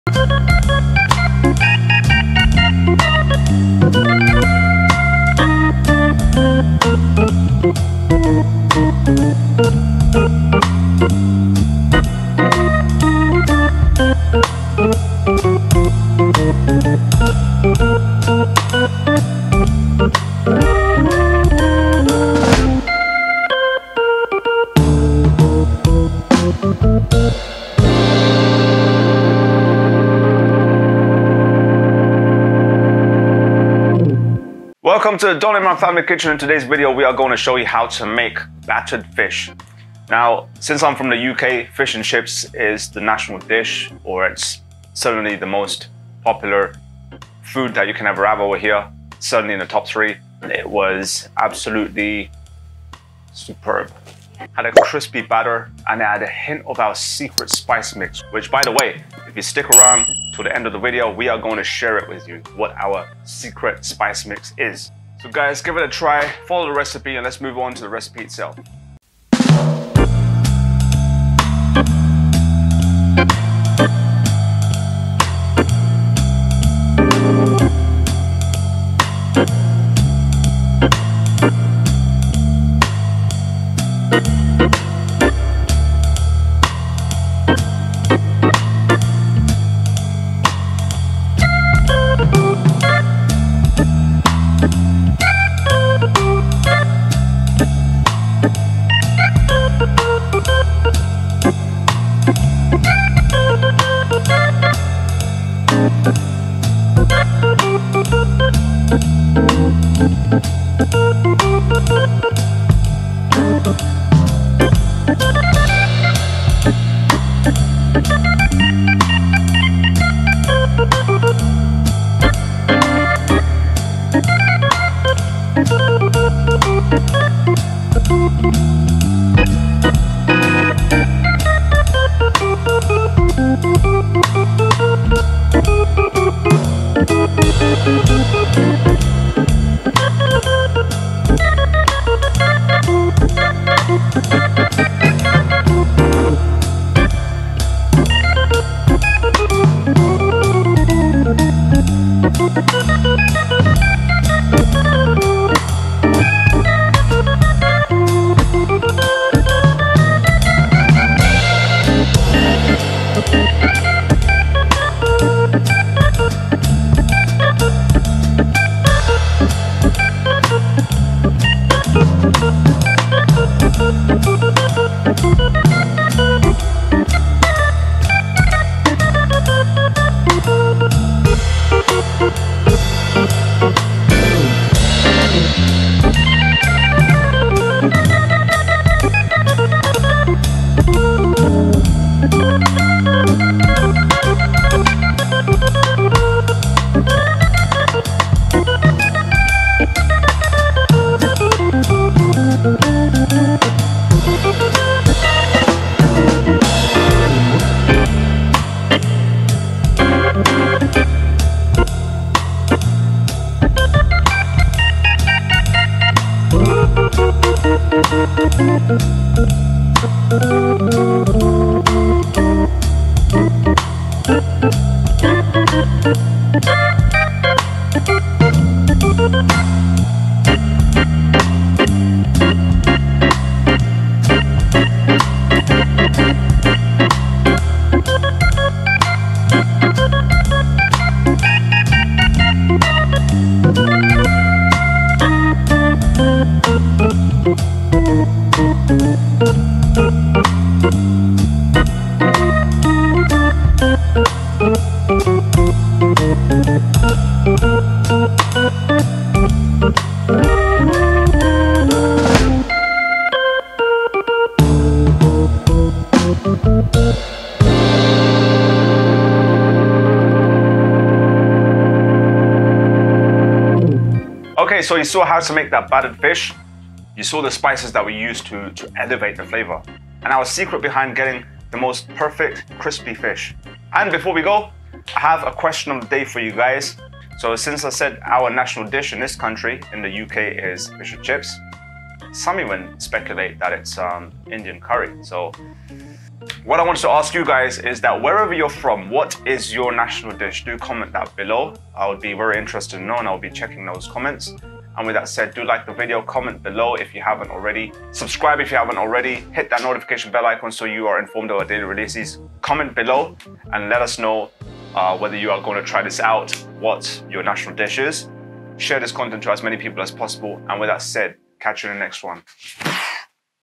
EASY BATTERED FISH RECIPE \\ SECRET BEHIND GETTING PERFECTLY CRISPY BATTER \\ COD WITH SECRET SPICE
In this video, we show you an Easy Battered Fish Recipe and share the secret behind getting a perfectly crispy batter for your cod. We have used Cod Fillets in this recipe and we also share the secret spice mix that goes into the fish batter, which gives you the wonderful subtly spiced fried fish. Fish and Chips is the national dish of the UK where I live, and since our school days we have had Battered Fish on most Fridays. My family and I absolutely love fried battered fish (especially when it's cod) and it is also the meal of choice on each visit to the seaside!

*Please note some corrections to the recipe below (marked with *)
To make the Easy Battered Fish Recipe batter, you will need:
1 cup all-purpose flour
1/2 cup cornflour (also known as corn starch)
salt to taste
1/2 tsp red chili powder
1 tsp paprika
1/4 tsp black pepper
1/2 tsp garlic powder
1/4 tsp baking soda
1 tsp baking powder
enough water to form a fairly thick batter
*1/2 tbs vinegar

For the Fried Fish flour mixture:
1/2 cup all-purpose flour
salt to taste
*1 tbs cornflour (also known as corn starch)

We used 4 x Frozen Cod fillets, but you can use haddock too.

The secret spice mix that goes into the batter gives a subtle wonderful seasoning to the fried fish. Some battered fish recipes use beer to create the bubbles, however we have shown a no beer version (alcohol-free) of getting the same bubbles to appear in the batter. The double-frying method also ensures the most wonderfully crispy Cod Fish for you to enjoy! We served our crispy battered codfish fillets with hand-cut chips and peas, with a selection of sauces.

The fish batter was extremely crispy and had the most wonderful seasoning. We hope you have seen how easy it is to make this battered fish recipe and the secret behind getting perfectly crispy cod fillets. We are sure you and your family will love trying out this secret recipe for crispy battered cod (or any fish of your choice).